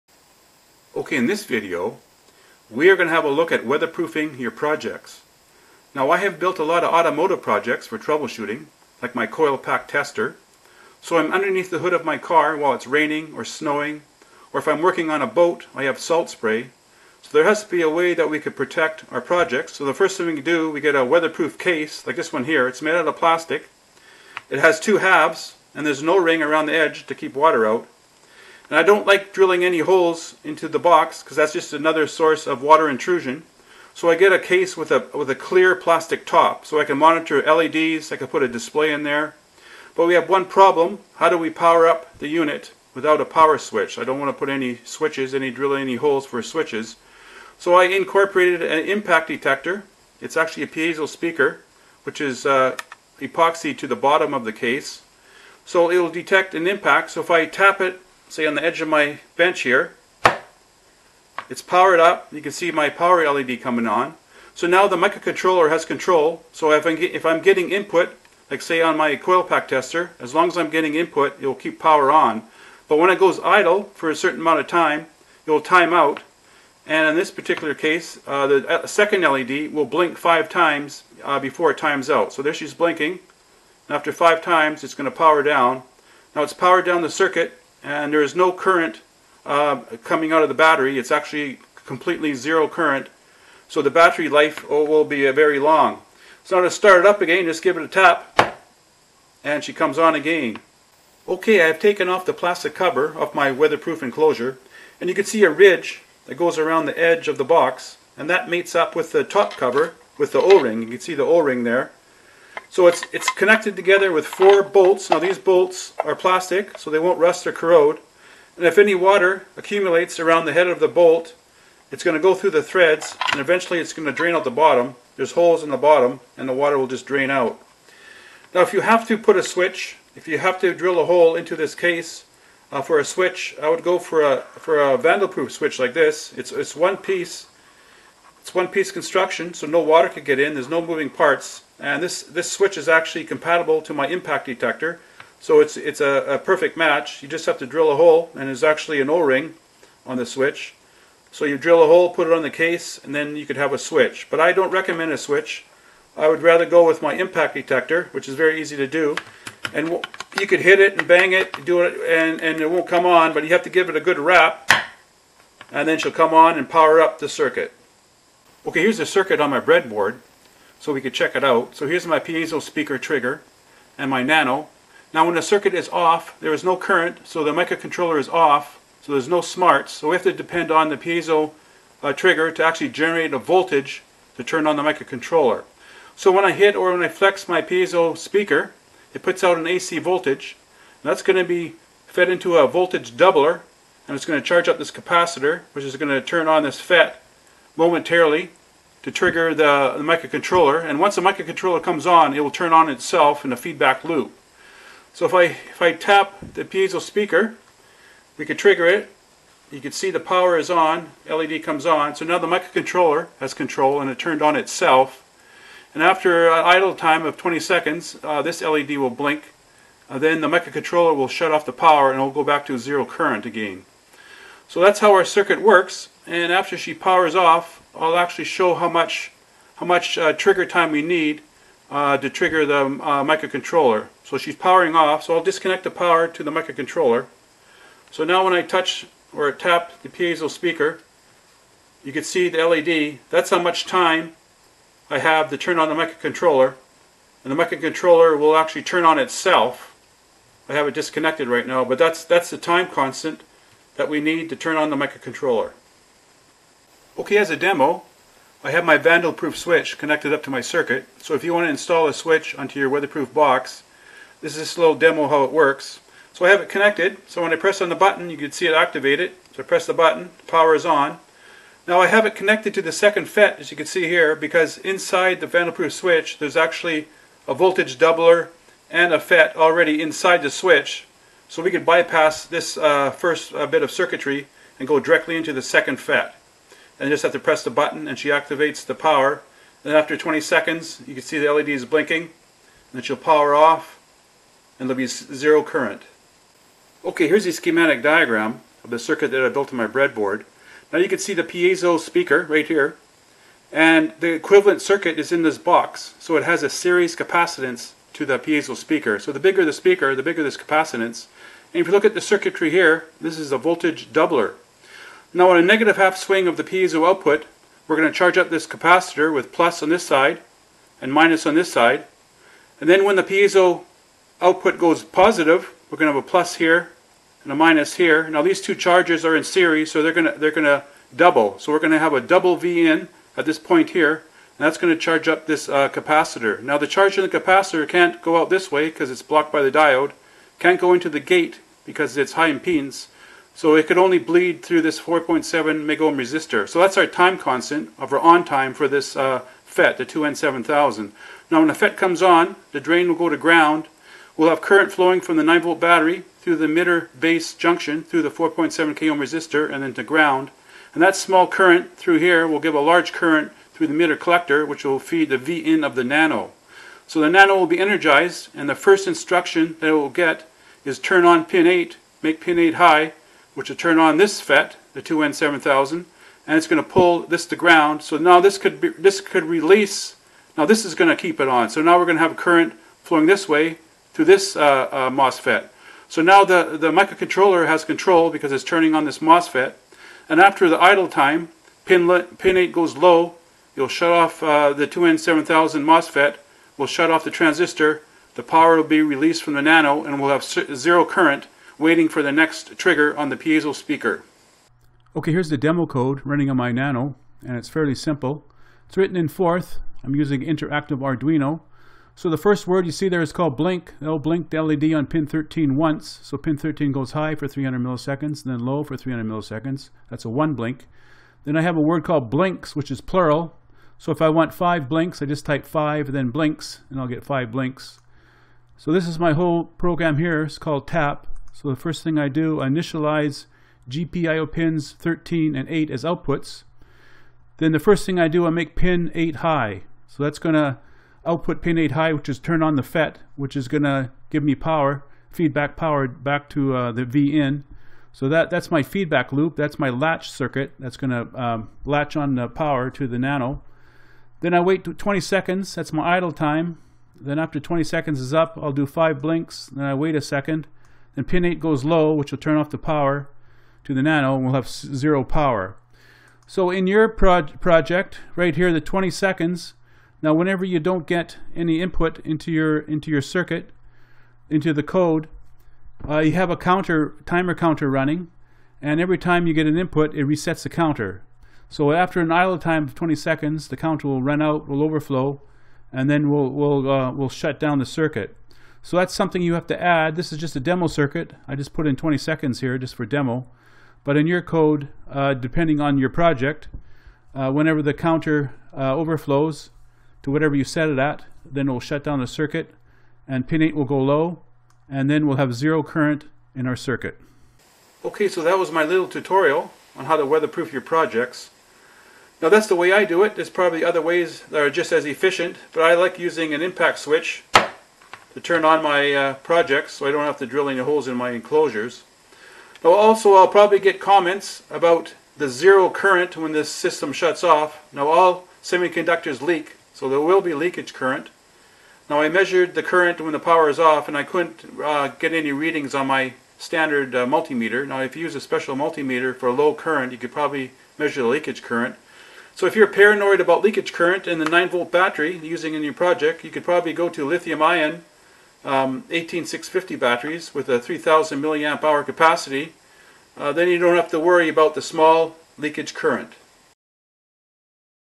How To Weatherproof Your Projects / Impact Power Switch / Forth
This video will give you some tips on how to weatherproof your projects. Circuitry will be covered on how to switch power to your projects without a physical power switch so no holes are needed in the enclosure.

Correction: Bipolar transistor is a PNP 2N3906